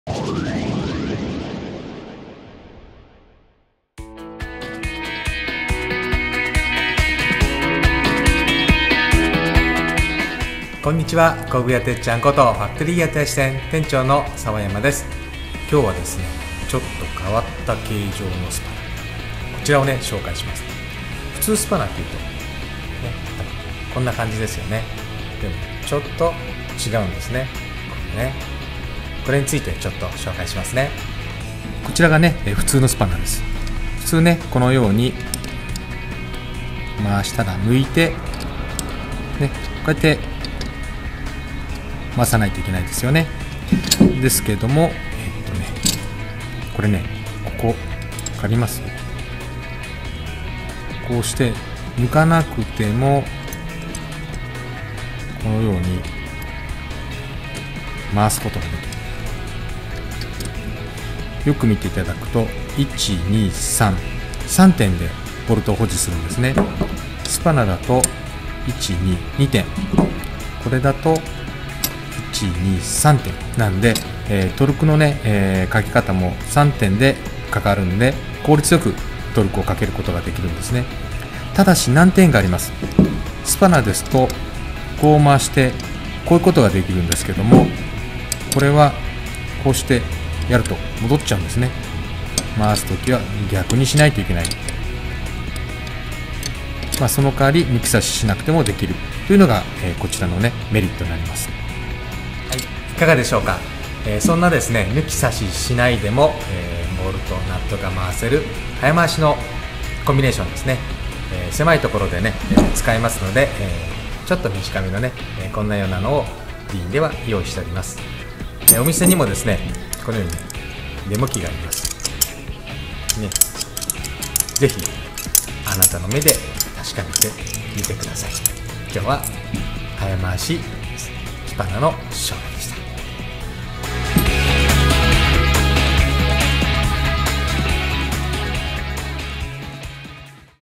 早回しが出来るDEENのコンビネーションレンチの紹介【ファクトリーギアの工具ブログ】
スパナって抜き差しして使いますよね。本来それでも早回しの工具の一つなのですが、そのスパナを抜き差ししなくても回せる早回しのコンビネーションレンチの紹介です。